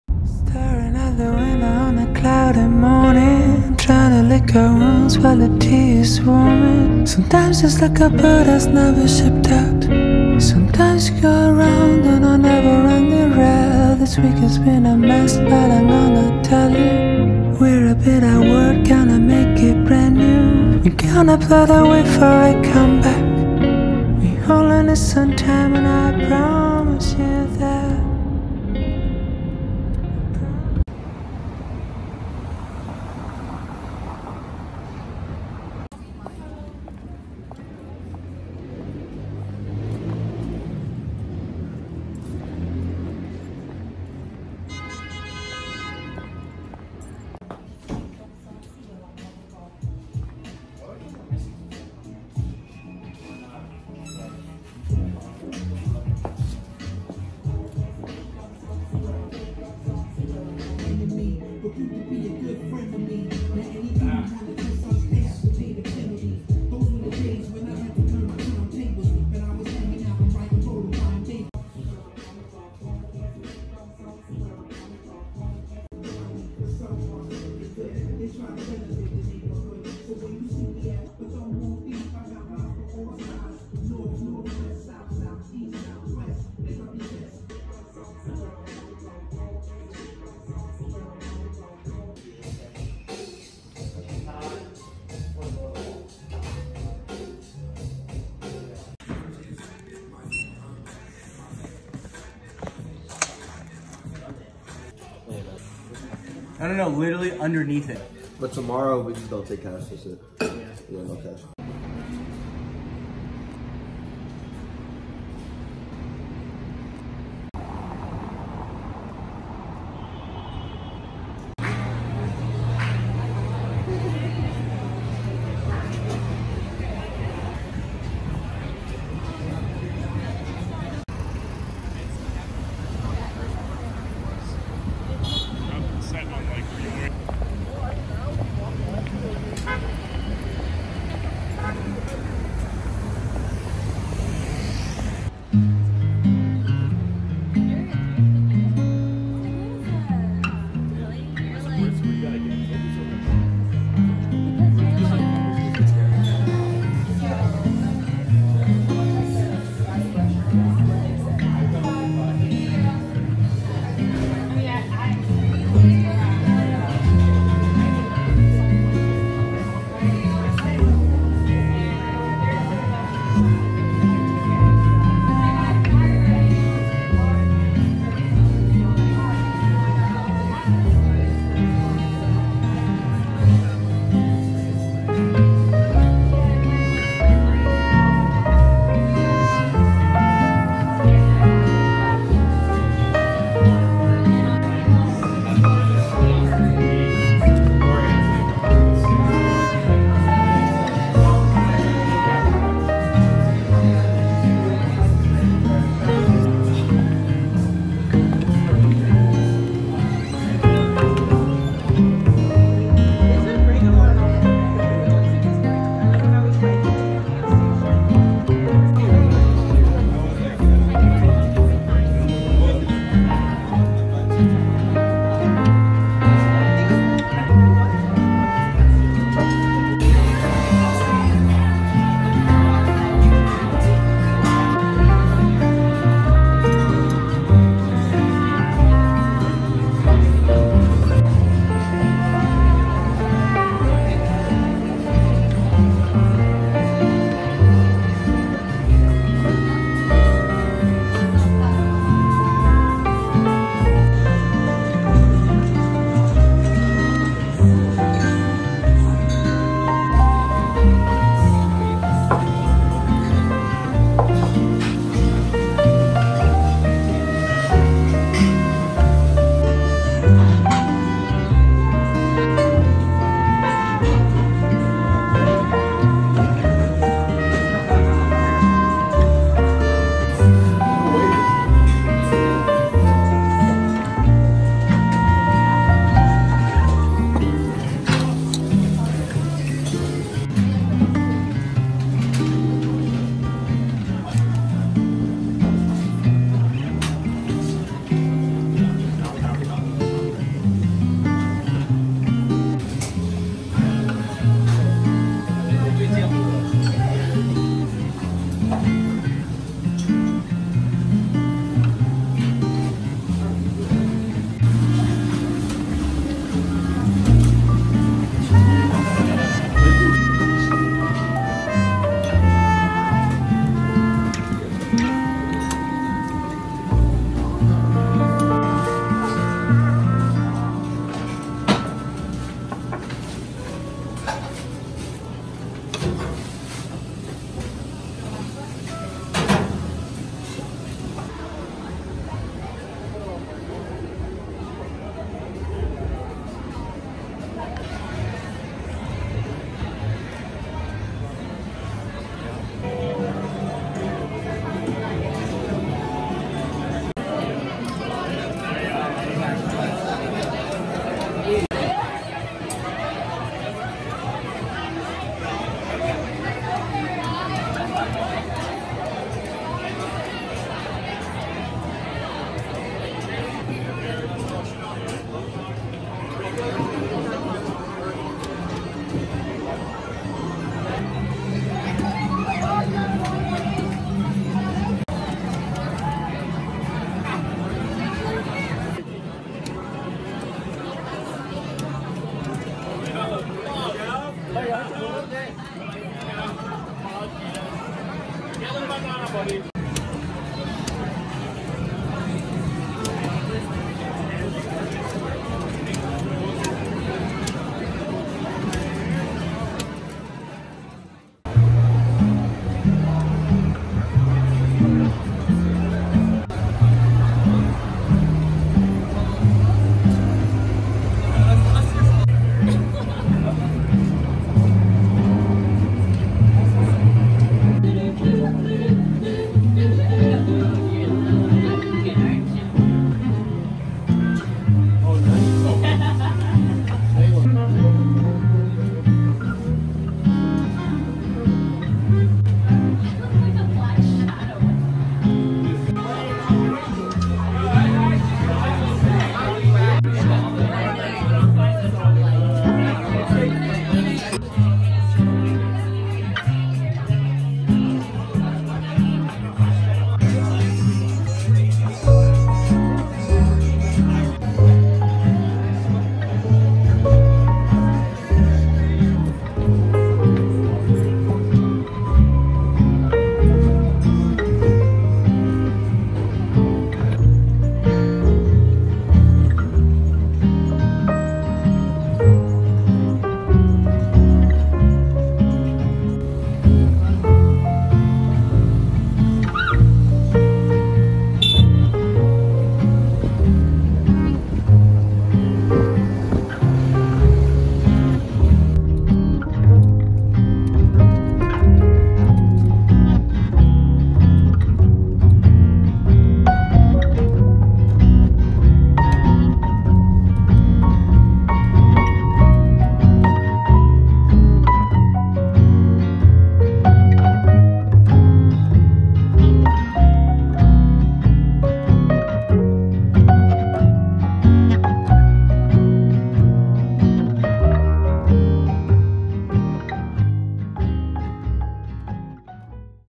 VLOG | 뉴욕 브이로그 | New York vlog | 뉴욕 이스트빌리지 맛집, Mara Project, Kimura, Snow Peak, Sake Bar 사케바
Thanks for visiting my channel, please enjoy!

* Pease set the video quality to 2160p 4K